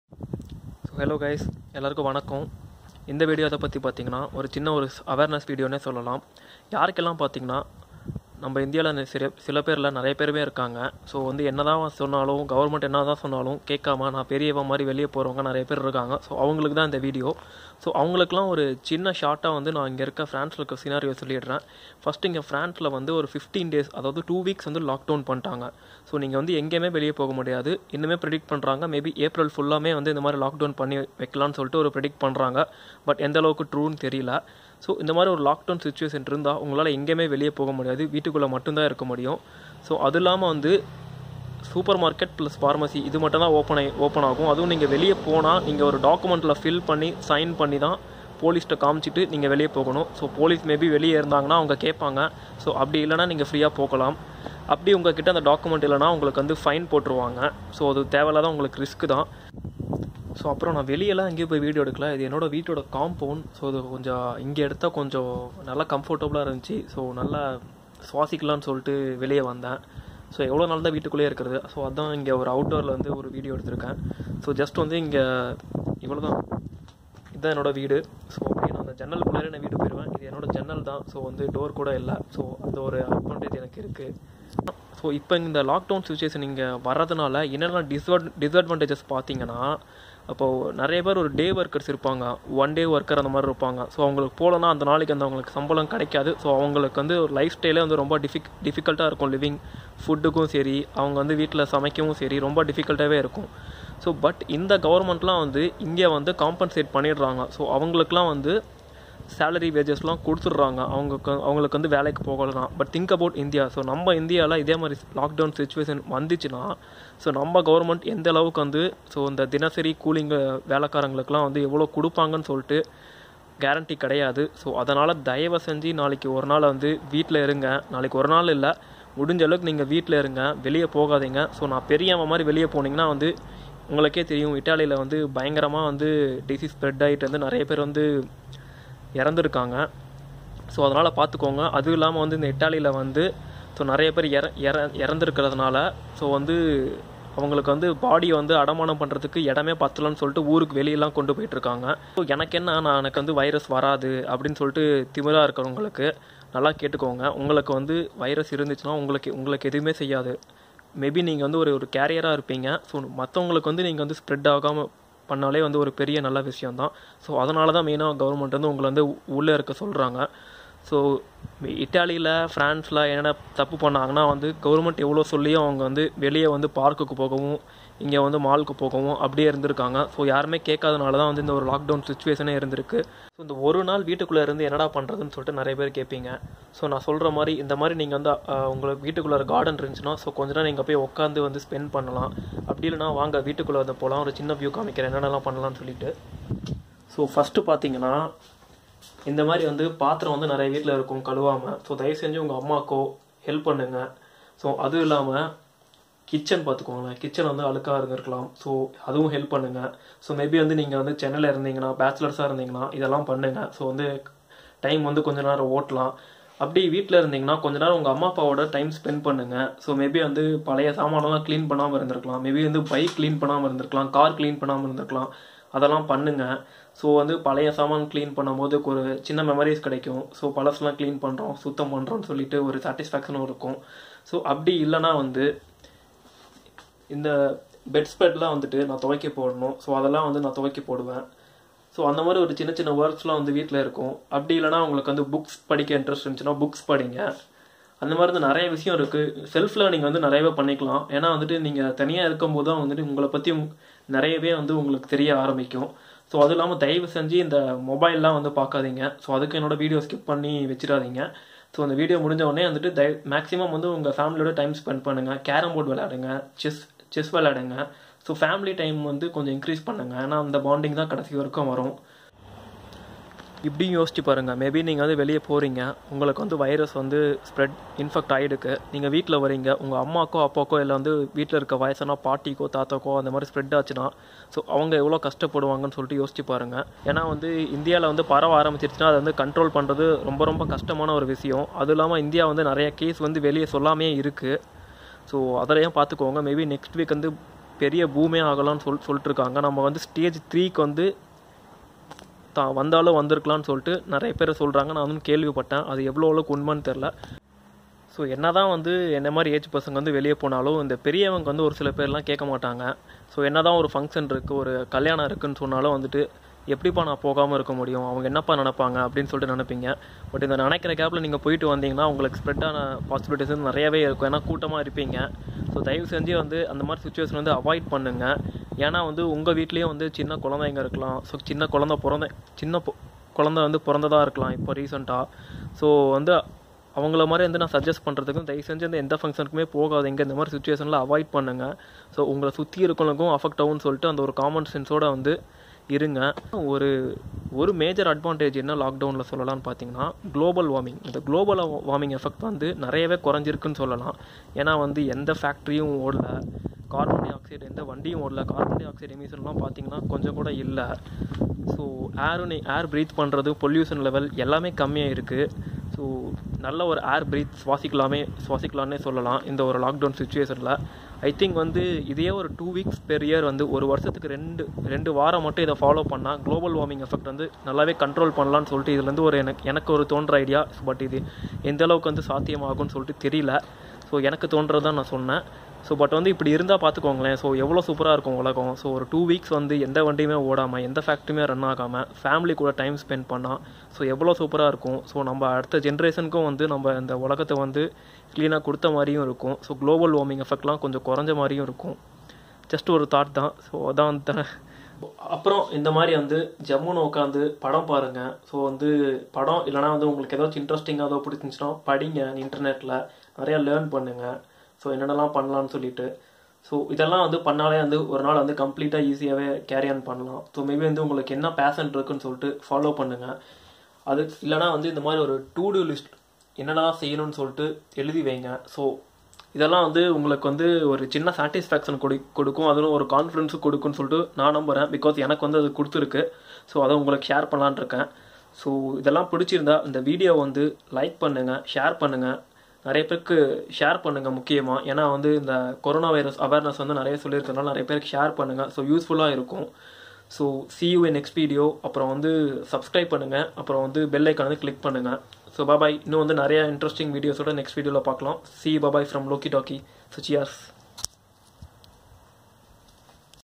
Lockdown | Staying away from everyone | How to spend time in home | Tamil | Loki Talkie
STAY HOME!!! PLEASE STAY HOME!!! Explained the lockdown situation in France and watch what we can do in one day at home. Highly Family Time!!!

With love from Loki!
See you in the next exciting video.